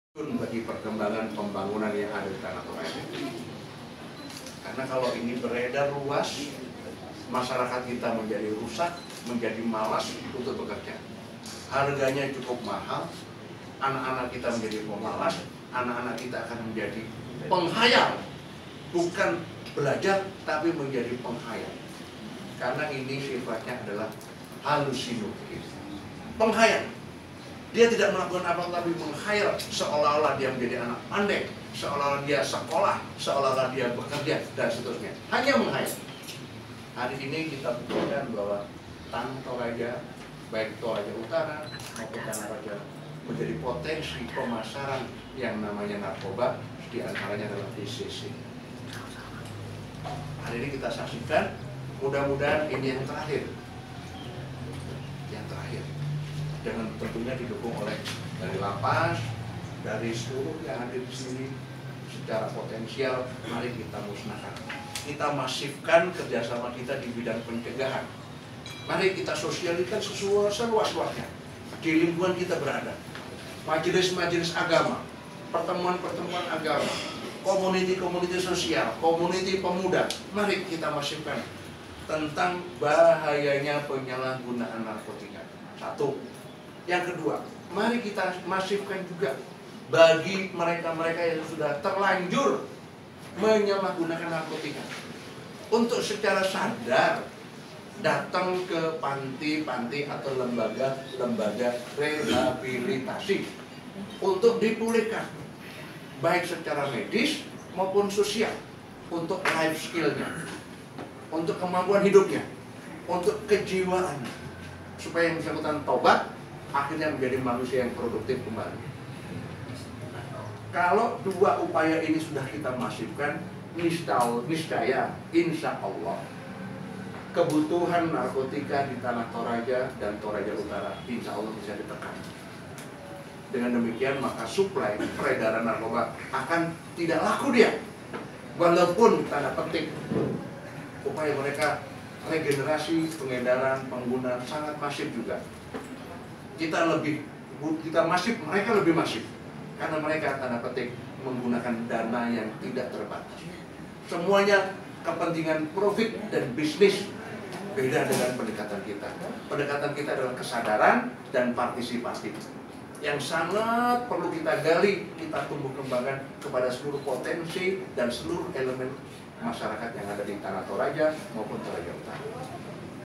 Kiat Kepala BNNP Sulsel Cegah Penyalahgunaan Narkoba di Toraja
Kepala BNNP Sulsel, Brigjen Pol Mardi Rukmianto mebgaja seluruh elemen masyarakat toraja untuk bergandengan tangan memberantas dan mencegah penyalahgunaan narkoba